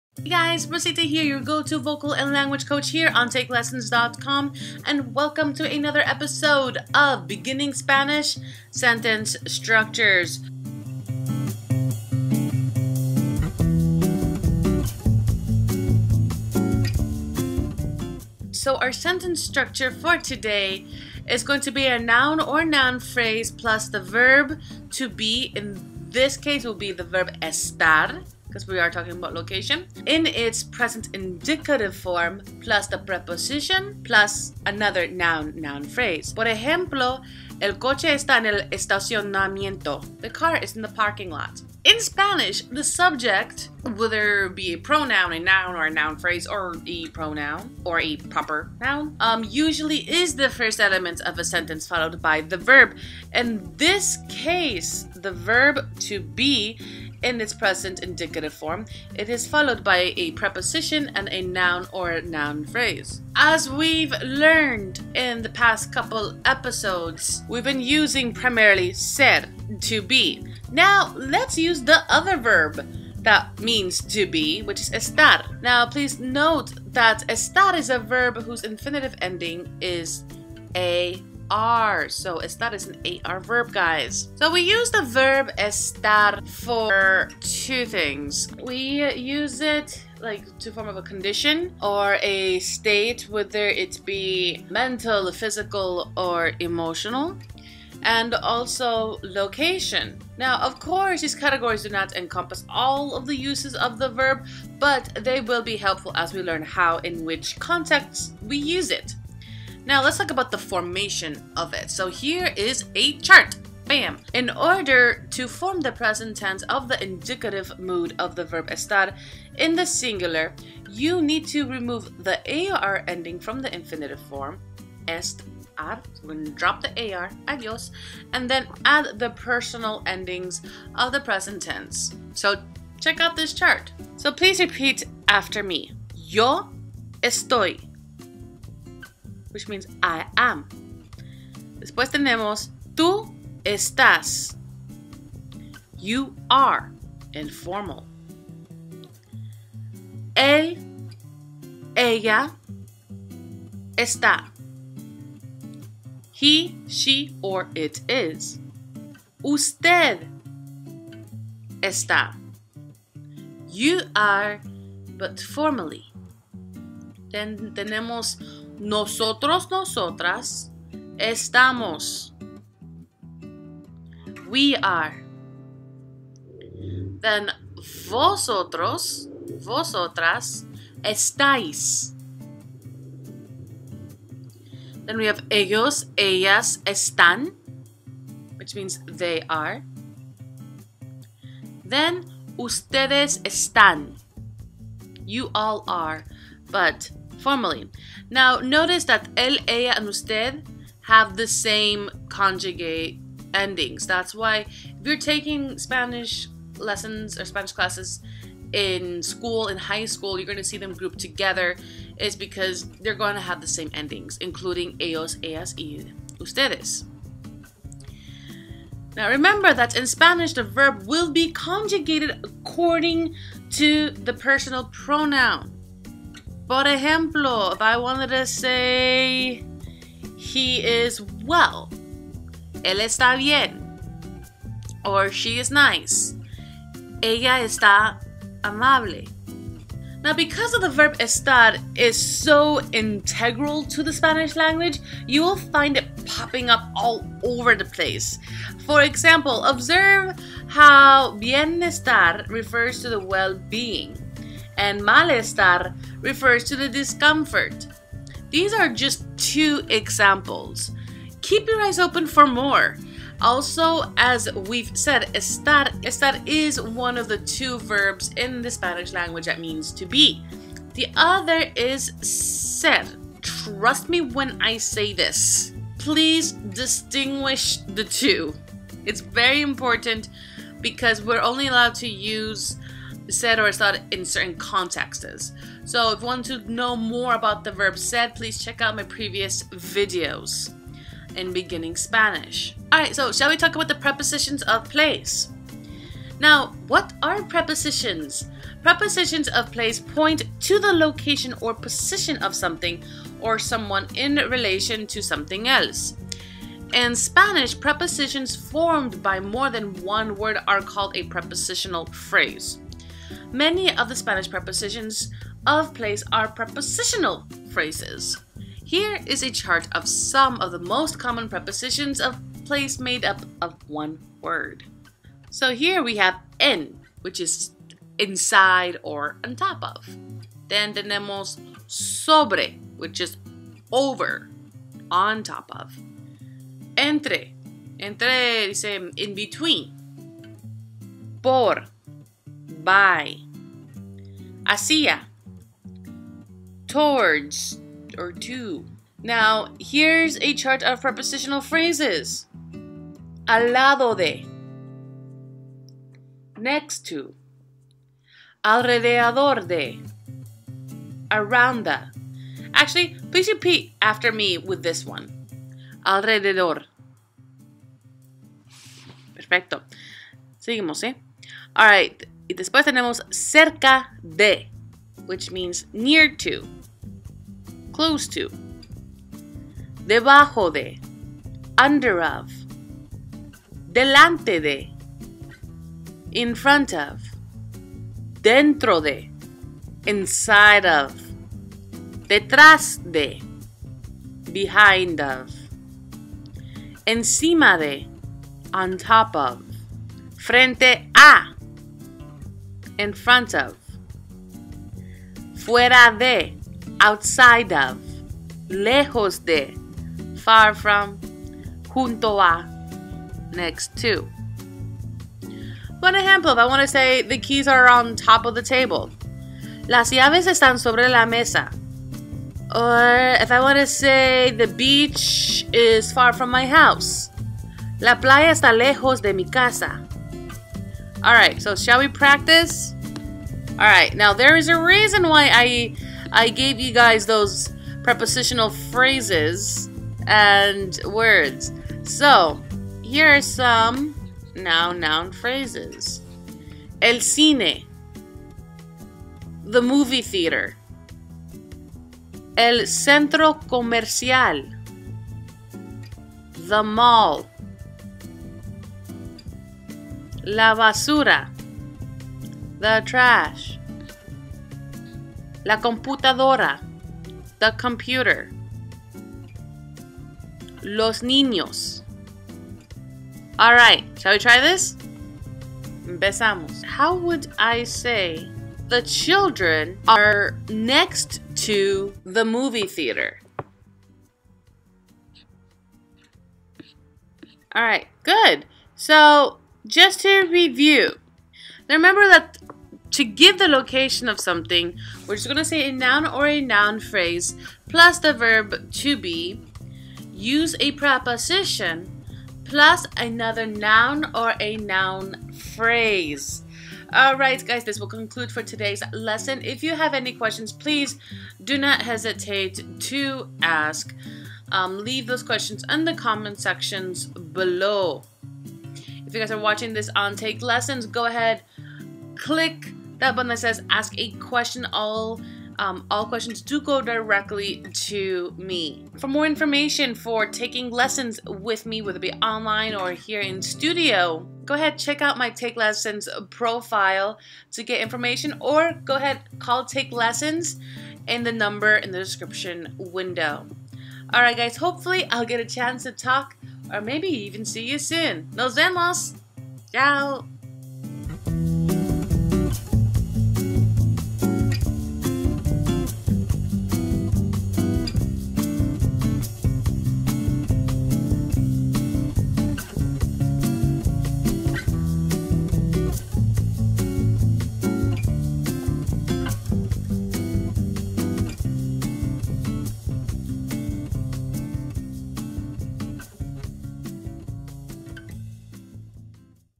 Beginning Spanish Prepositions
BEGINNING SPANISH- SENTENCE STRUCTURES-Prepositions

In this lesson, we will learn how to  give the Location of Something using Prepositions! Also how to use the verb "Estar" when talking about location.

For information on private and group lessons, in-studio or online check out